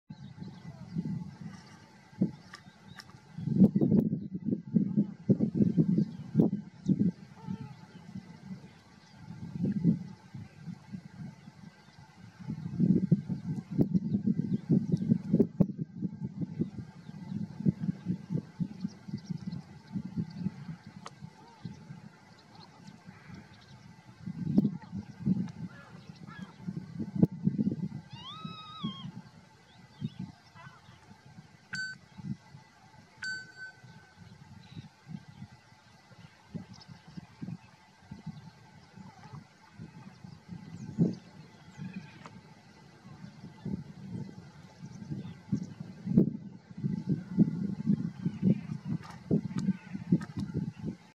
Gaviota cana (Larus canus)
Gaviota cana en el Paraje Natural del Brazo del Este (Sevilla)